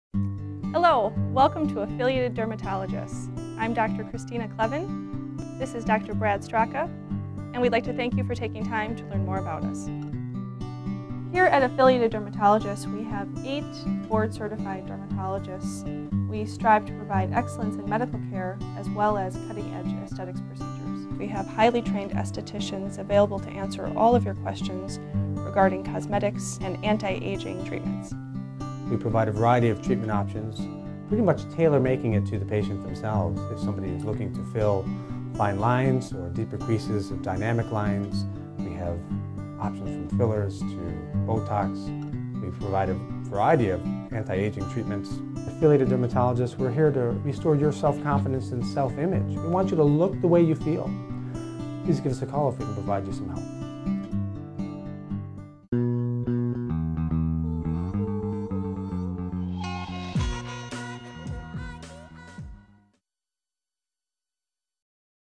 Brookfield Skin Care Affiliated Dermatologists, SC
Reclaim your youthful looks. Come to Affiliated Dermatologists in Brookfield, Wisconsin. This advanced medical clinic staffs a team of eight board certified dermatologists, supported by skilled and experienced estheticians. They offer a variety of proven techniques that'll smooth the wrinkles away. Make an appointment to learn more.